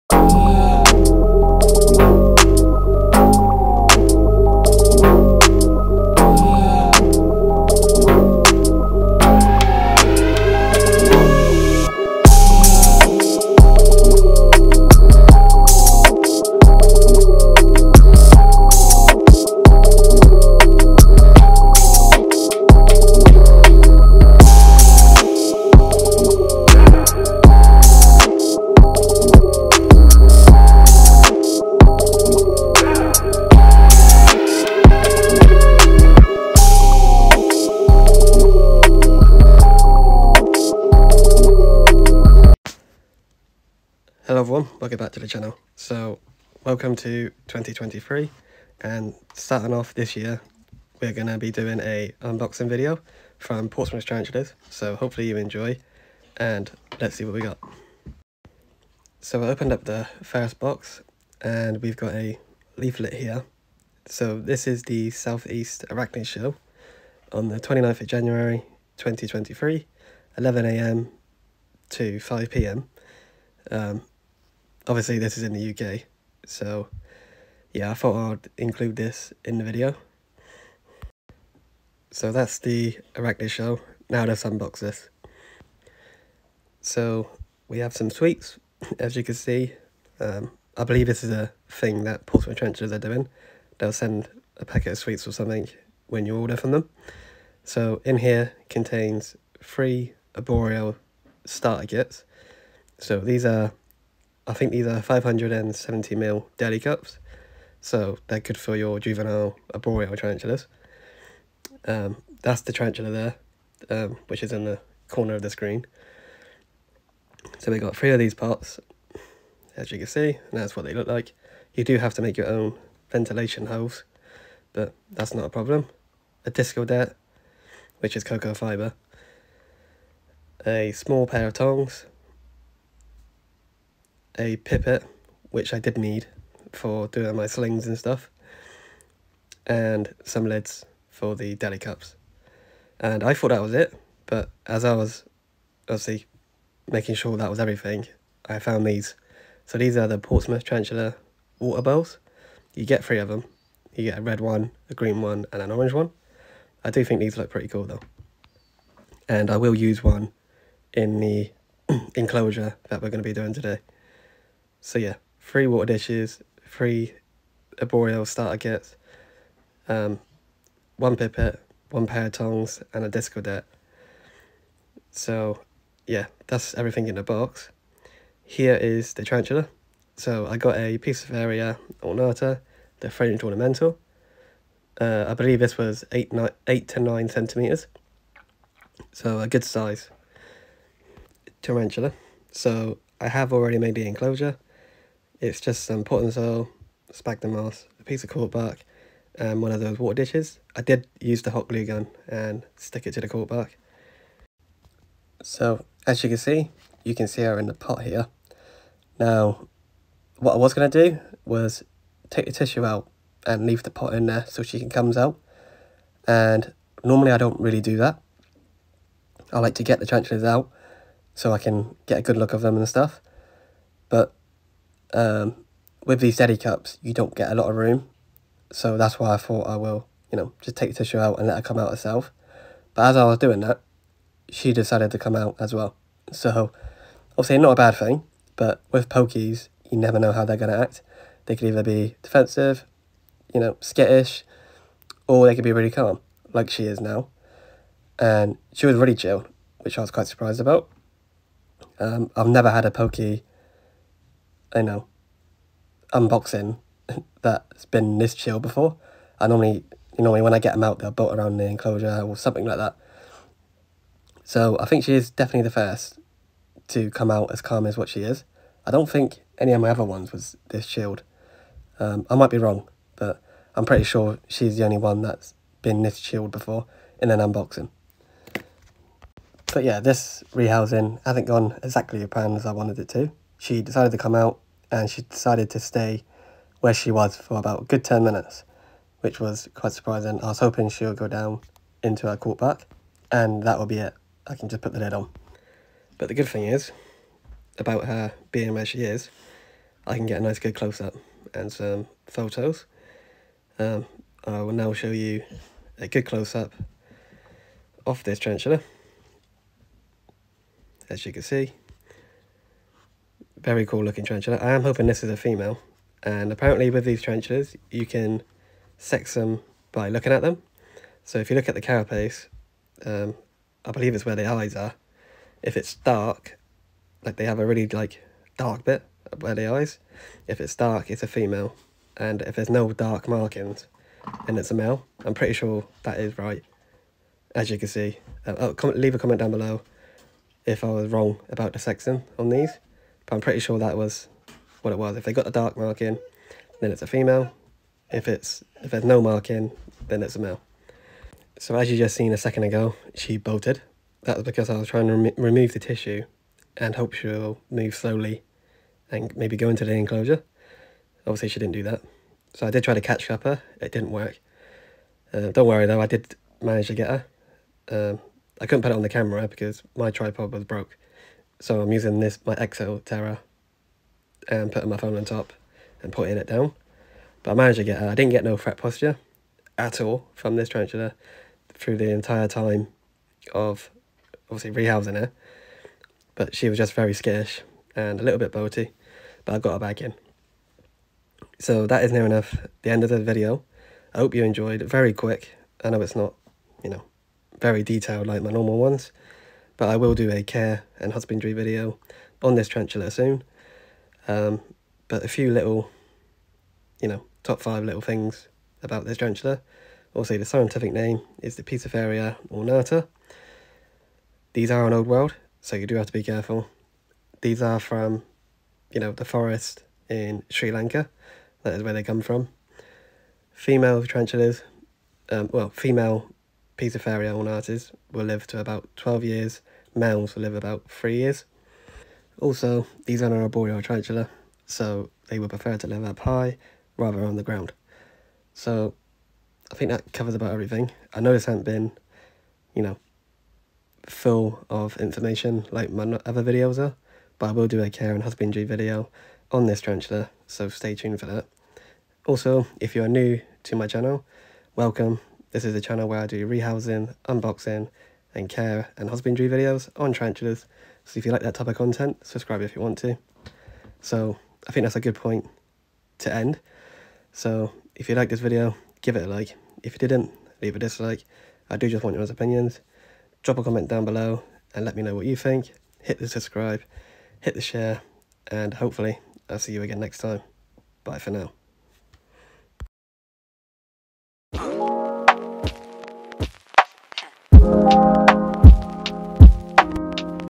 First unboxing 2023!
hello welcome back to the channel and to start of this year will be a unboxing from portsmouth tarantulas so i hope you enjoy

Musician: Rook1e